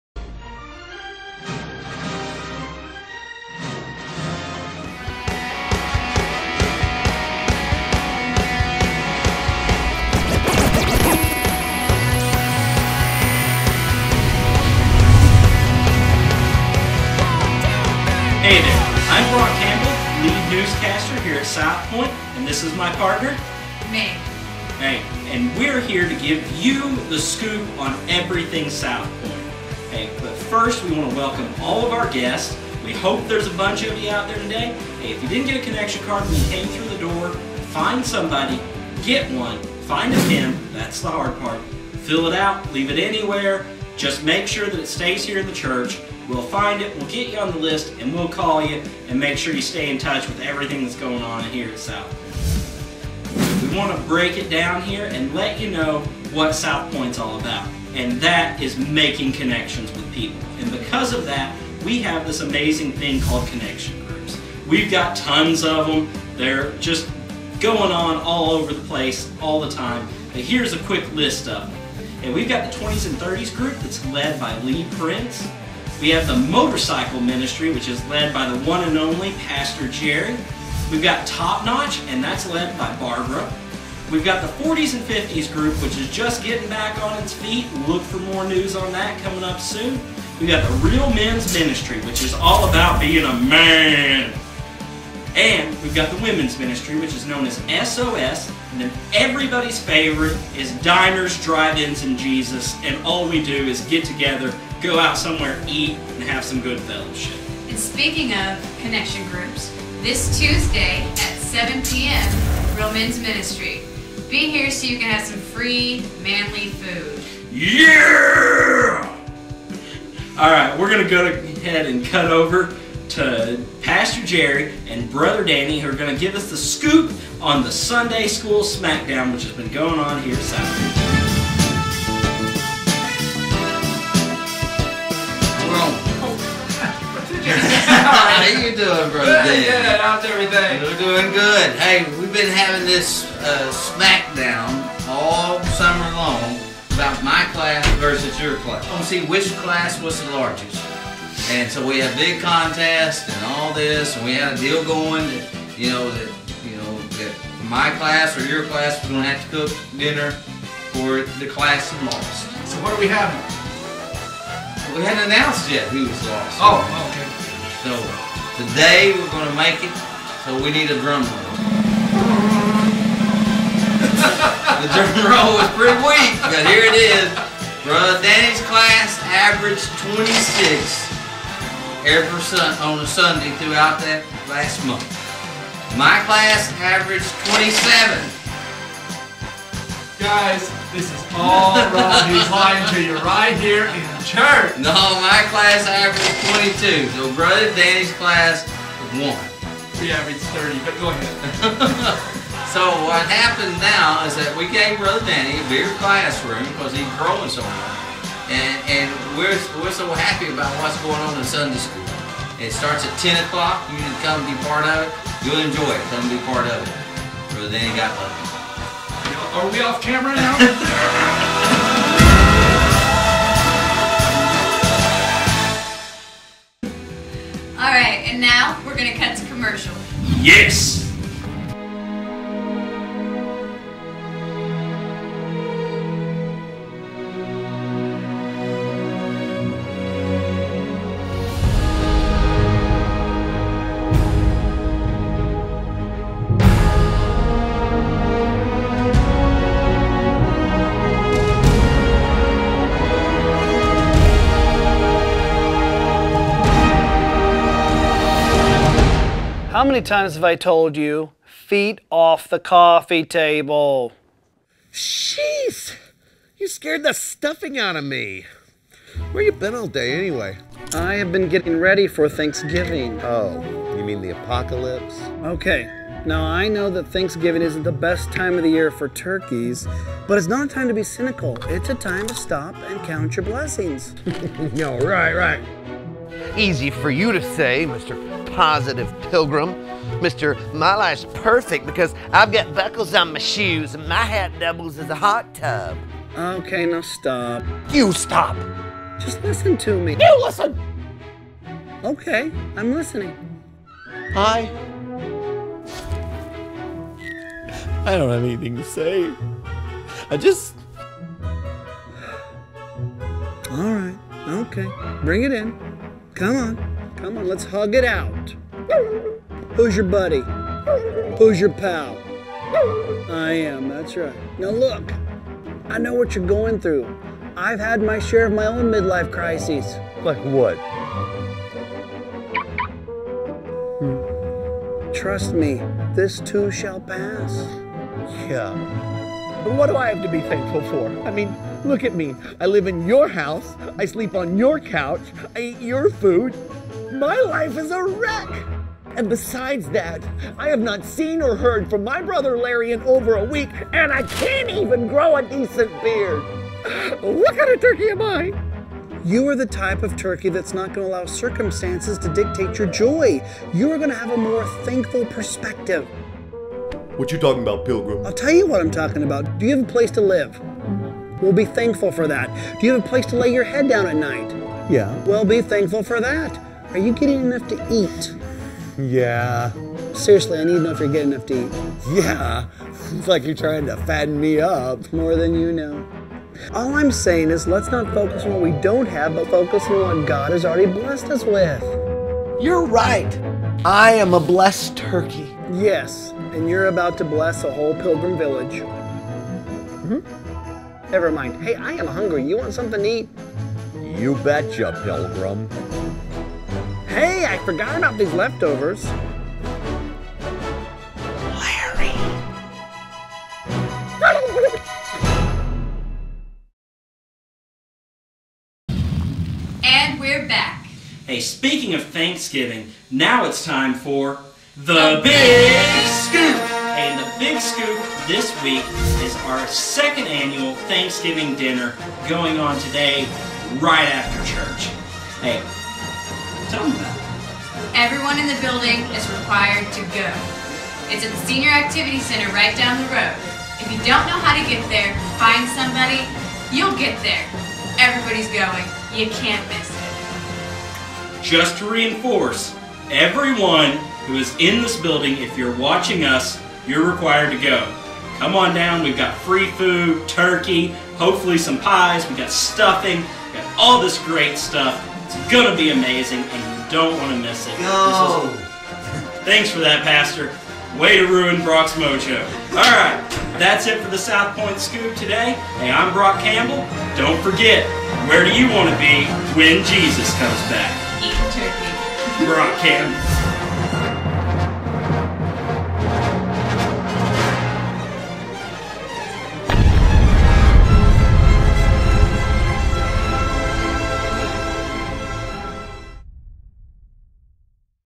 Nov 14 Southpointe Scoop Thanksgiving Celebration Edition- Counting your blessings 2
This is the Thanksgiving Celebration Scoop from Southpointe..We had 180 people attend church this morning..a record high for Southpointe, We hope to see you back next week!